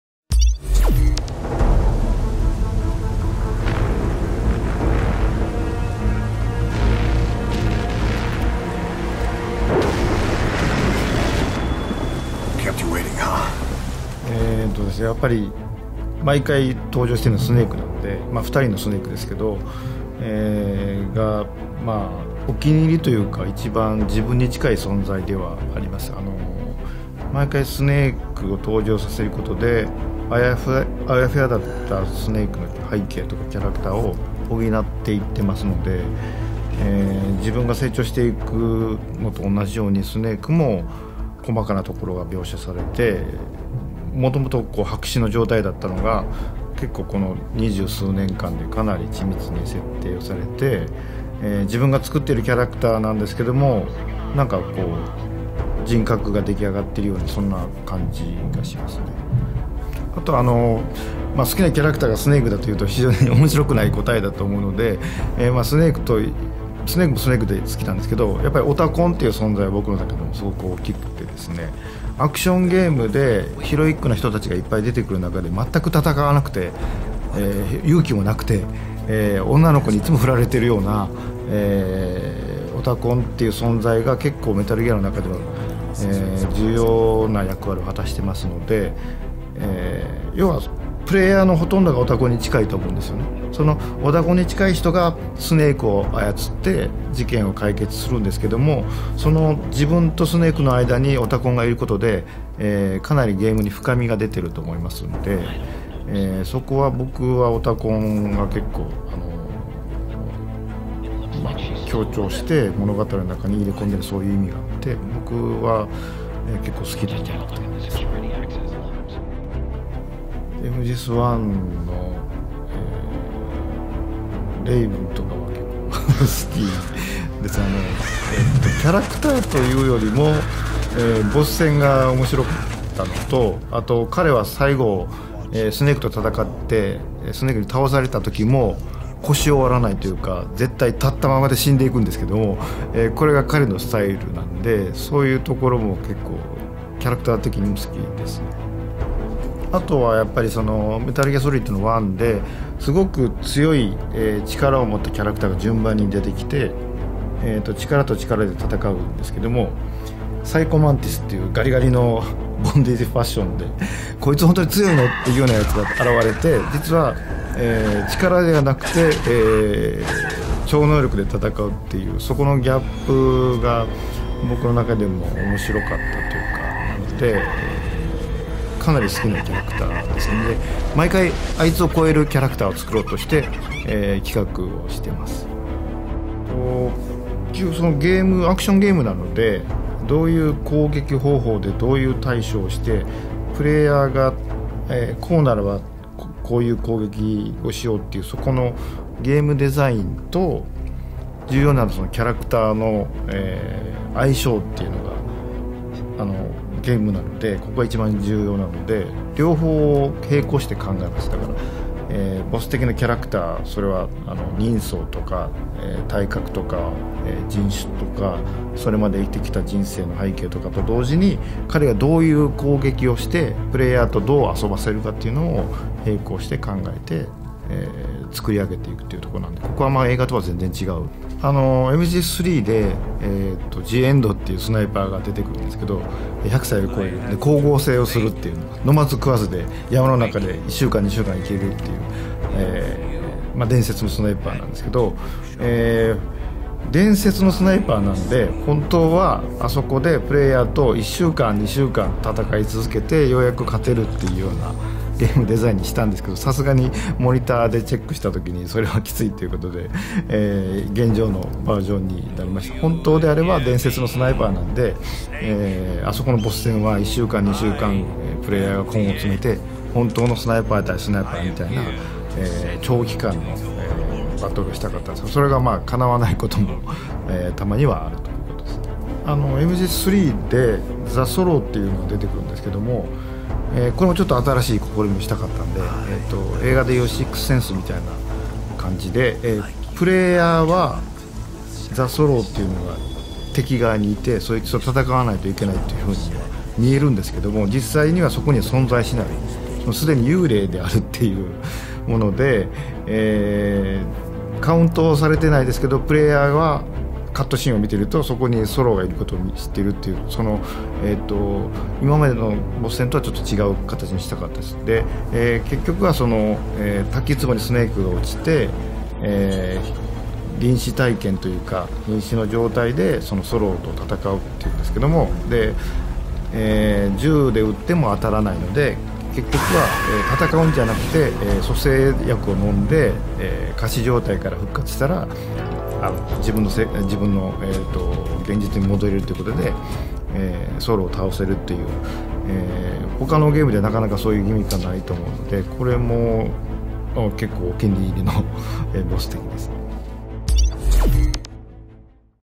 Hideo Kojima's Top 5 Metal Gear characters
From Snake to The End, the creator of Metal Gear Solid chooses his favourite characters from the series.